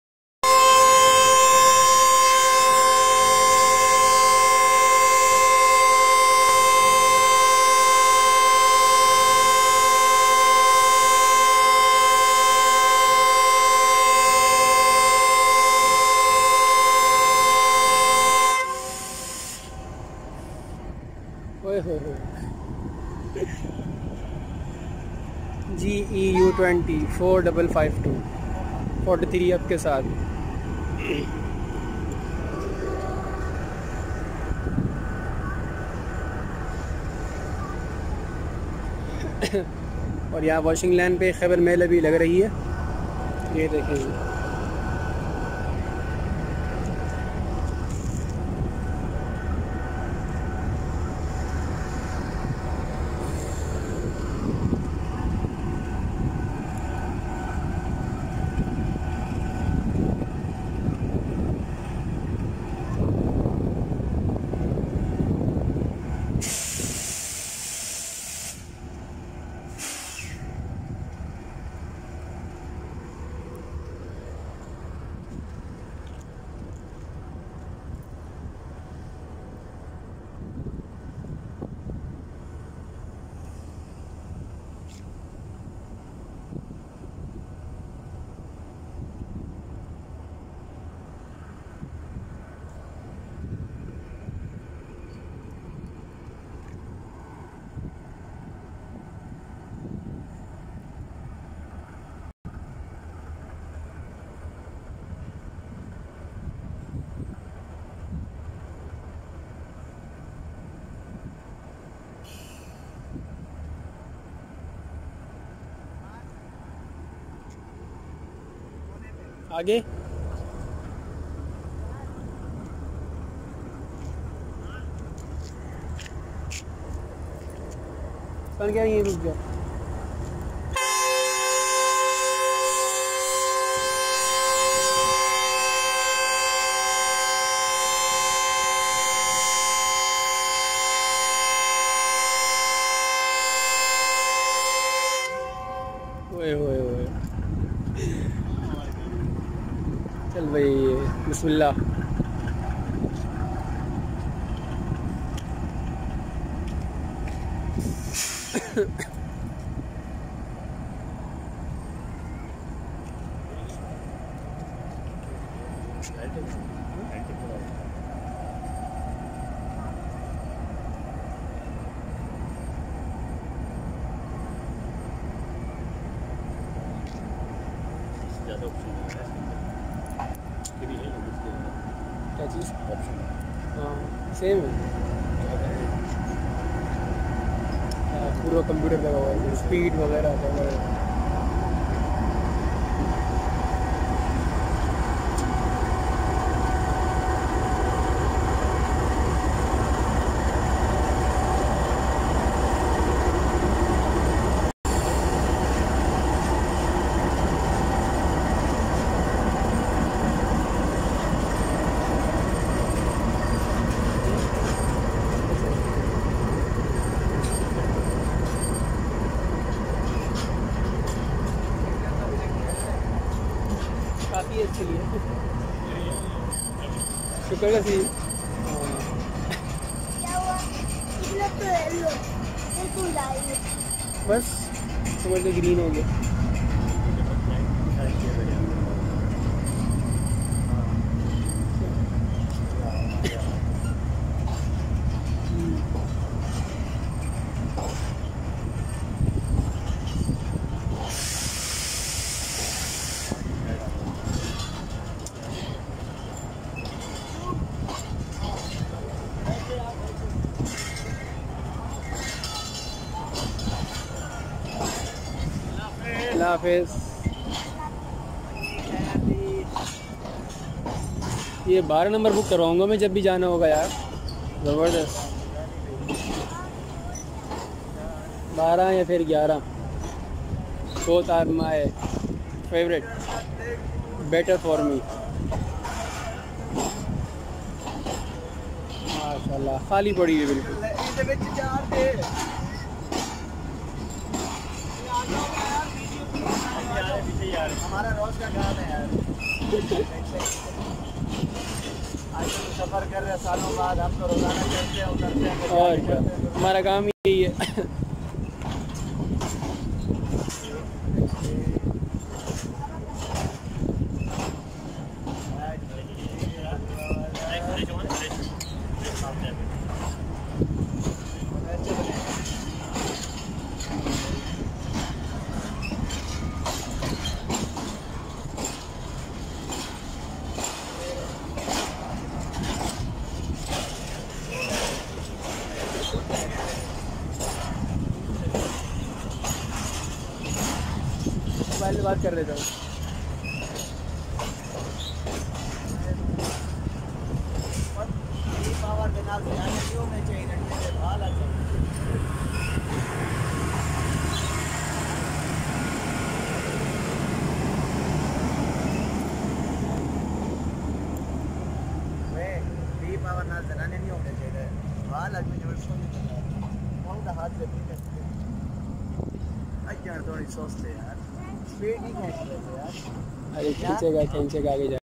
43up Shah Hussain Karachi Cantt departure || Long Whistle by GEU2O-4552 || Ammar P.R KC
43up Lahore bounded Shahhussain ( OLD Night CoAch ) leaving Karachi Cantt with Newly American Locomotive 2OOO HP GEU2O.
ttractive Night View Platform #08 Karachi Cantt.

Video bY "" Muhammad AmmAr""
Date :
Day :
Time :
Camera : Huawei Y9Prime 2019